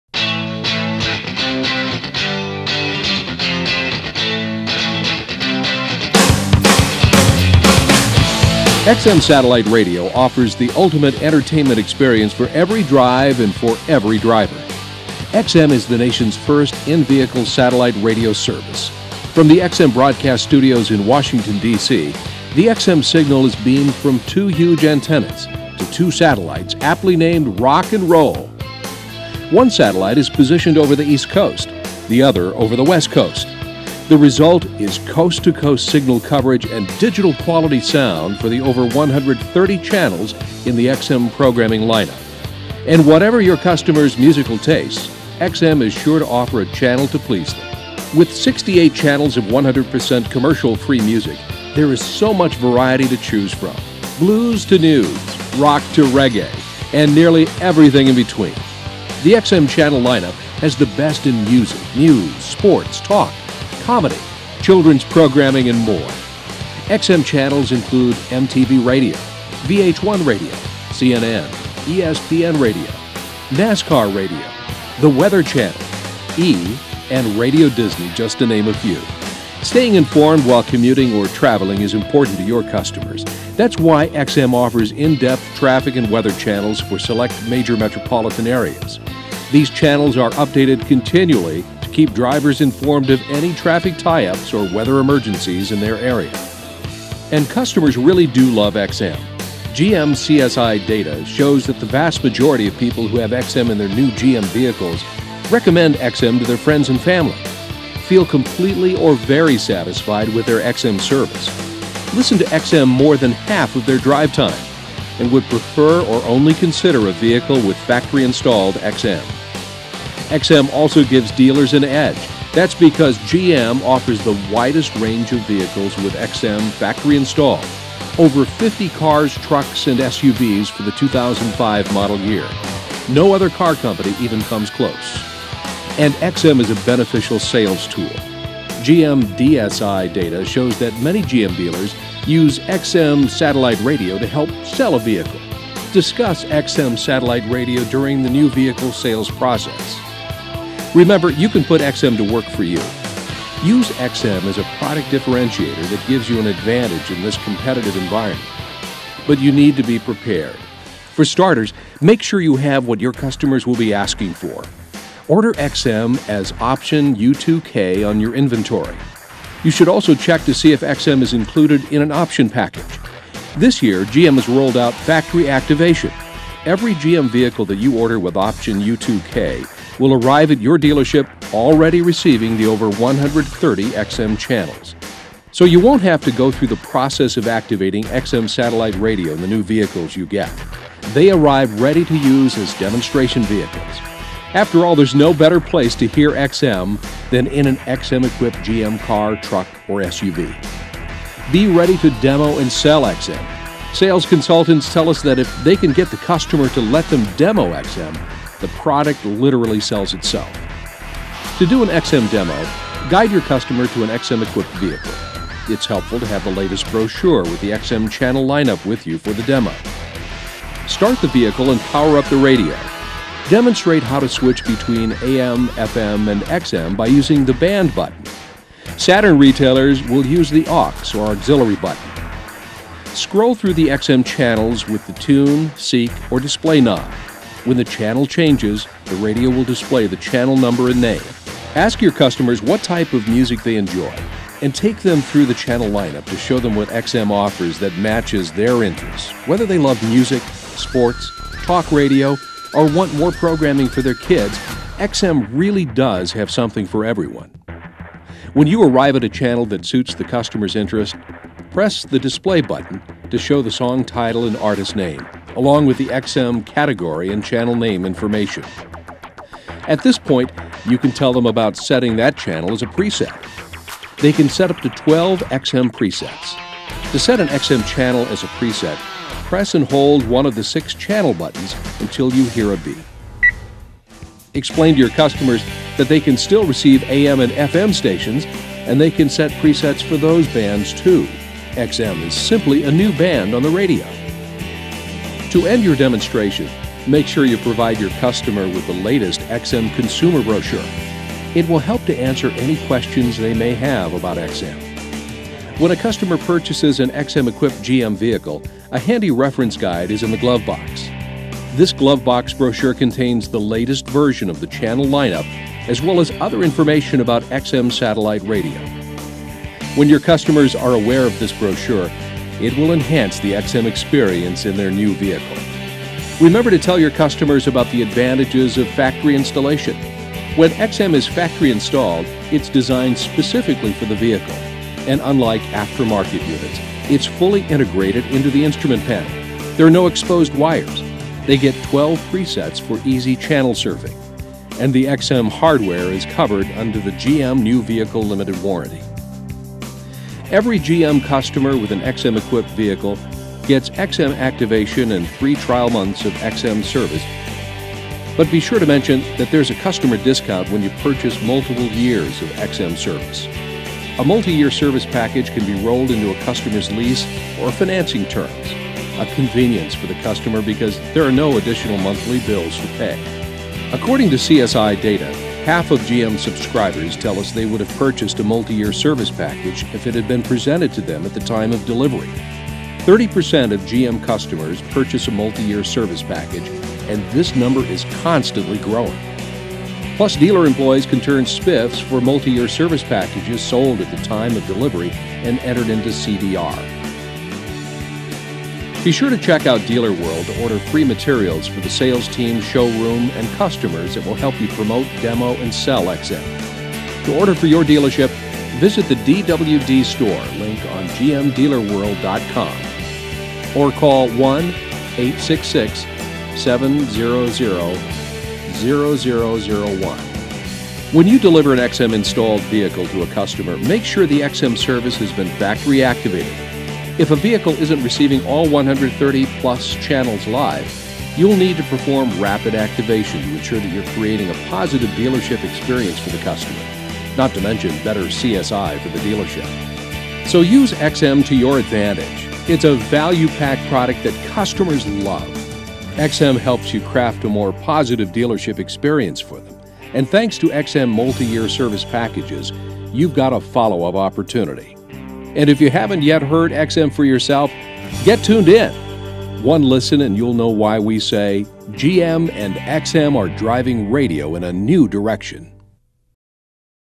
XM Satellite Radio for 2005
This Special Update from the 2005 GM Product Training Video Series discusses the XM Satellite Radio system available for 2005 GM vehicles. Topics include: demonstration tips, reference guide usage, factory installation features, and activation benefits.